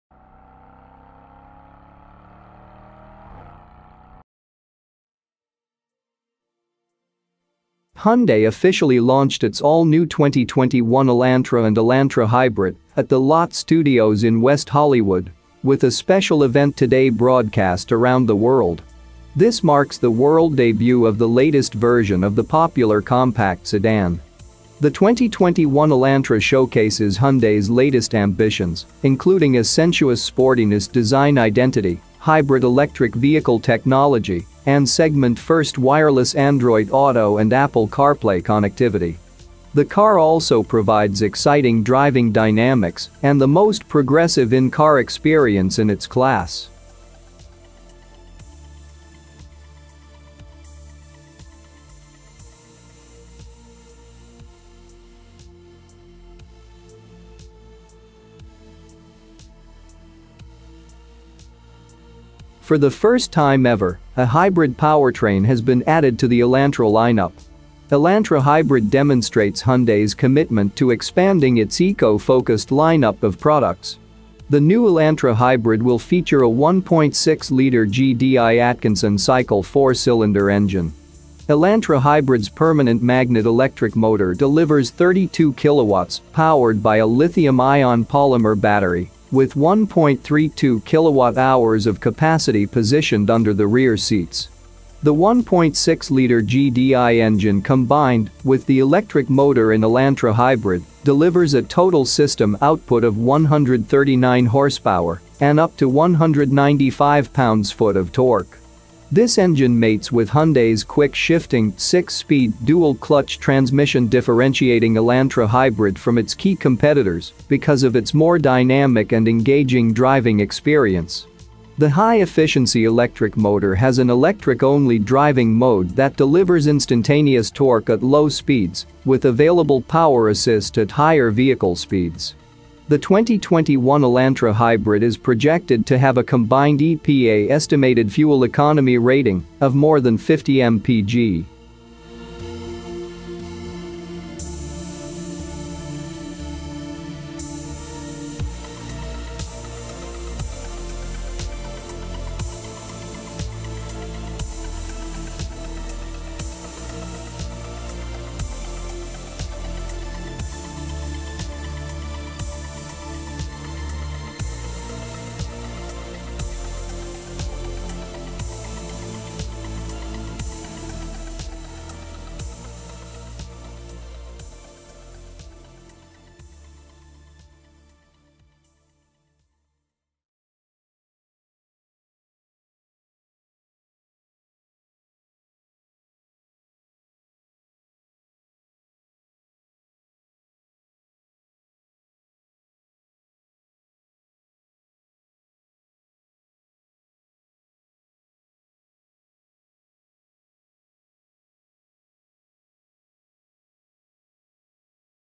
2021 Hyundai Elantra Hybrid Fuel Efficient New Compact Sedan
The new Elantra Hybrid will feature a 1.6-liter GDI Atkinson-cycle four-cylinder engine. Elantra Hybrid’s permanent-magnet electric motor delivers 32 kW powered by a lithium-ion-polymer battery with 1.32 kWh of capacity positioned under the rear seats. The 1.6-liter GDI engine combined with the electric motor in Elantra Hybrid delivers a total system output of 139 horsepower and up to 195 lb-ft of torque.